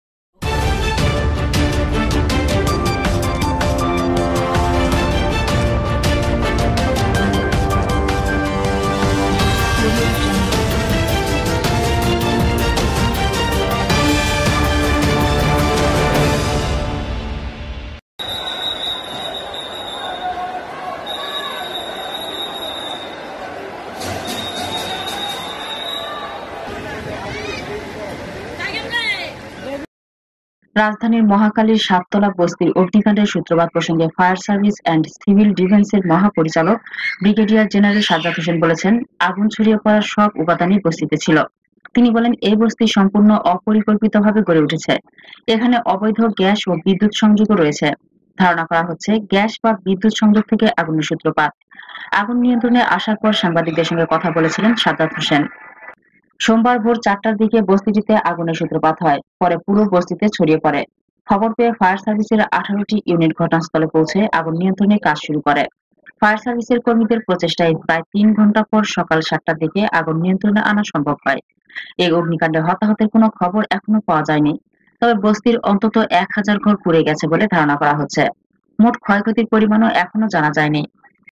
রাজধানীর মহাখালীর সাততলা বস্তিতে অগ্নিকাণ্ড|a2znewslivetv
রাজধানীর মহাখালীর সাততলা বস্তির অগ্নিকাণ্ডের সূত্রপাত প্রসঙ্গে ফায়ার সার্ভিস অ্যান্ড সিভিল ডিফেন্সের মহাপরিচালক (ডিজি) ব্রিগেডিয়ার জেনারেল সাজ্জাদ হোসেন বলেছেন, আগুন ছড়িয়ে পড়ার সব উপাদানই বস্তিতে ছিল।তিনি বলেন, ‘এই বস্তি সম্পূর্ণ অপরিকল্পিতভাবে গড়ে উঠেছে। এখানে অবৈধ গ্যাস ও বিদুৎ সংযোগও রয়েছে। ধারণা করা হচ্ছে- গ্যাস বা বিদুৎ সংযোগ থেকে আগুনের সূত্রপাত।